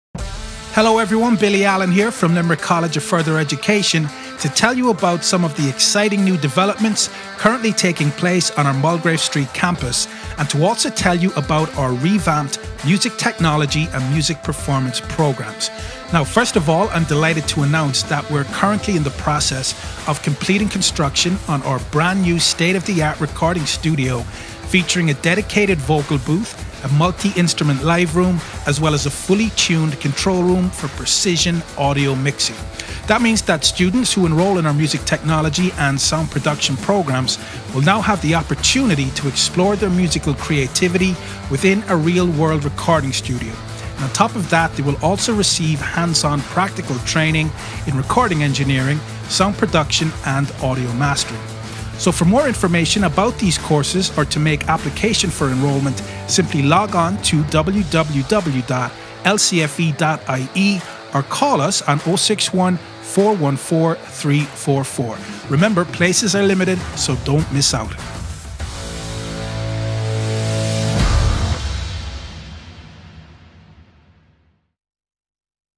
Music Technology & Sound Production - Limerick College of Further Education
This 1 year, full-time programme is suitable for learners interested in a career in radio, music, sound engineering, music promotion and or professional DJ’ing. Learners are given the opportunity to pursue a career in DJ’ing,radio production and sound engineering.The course aims to provide the learner with the skills to develop their own natural abilities in music production performance and radio broadcasting